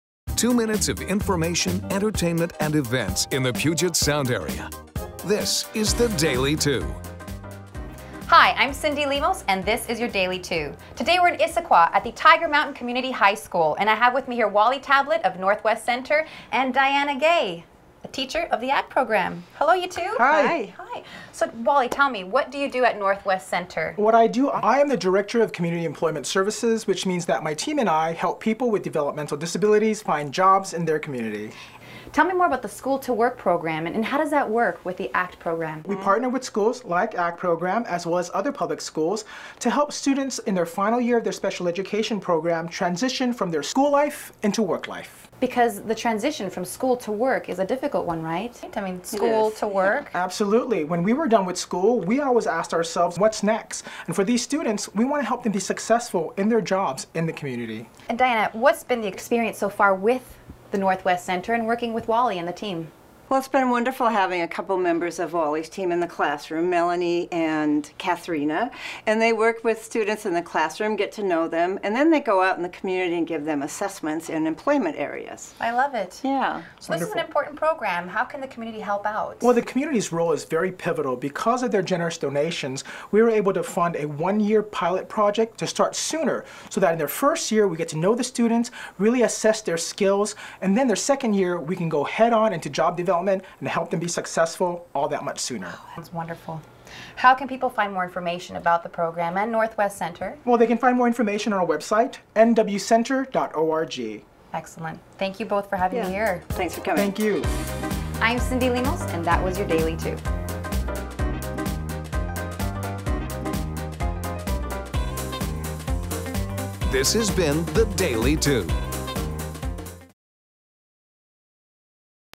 Northwest Center School-To-Work Program - Produced by KIRO 7 TV
Every year, Northwest Center works with students graduating from high schools in King County as they seek to transition into the workforce. Utilizing our vast array of contacts and connections we have enjoyed great success in finding jobs for these students so that they leave school with a job and enter the working world with confidence.